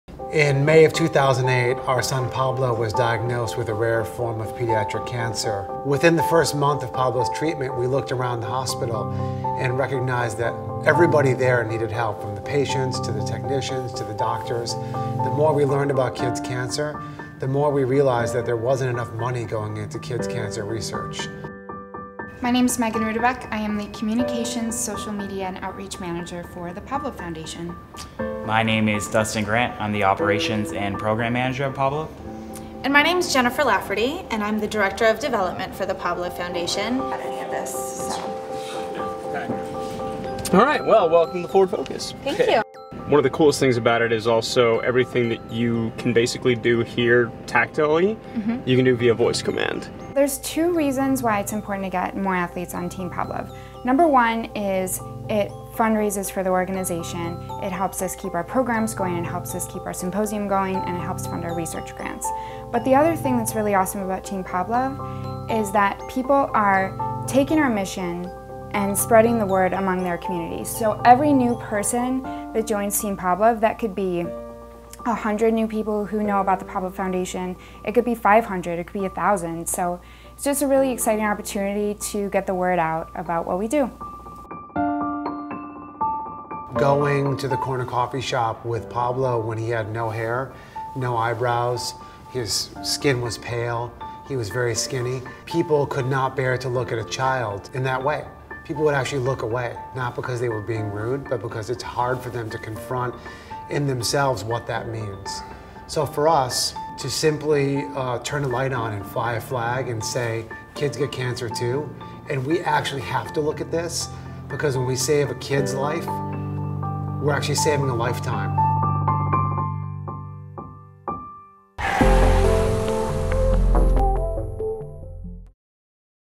The Pablove Foundation - Episode 1
Meet the people that are fighting childhood cancer with love at the Pablove Foundation.  This summer, follow their story as they try to get 46 athletes onto Team Pablove.  Join them and help spread the message that kids get cancer too.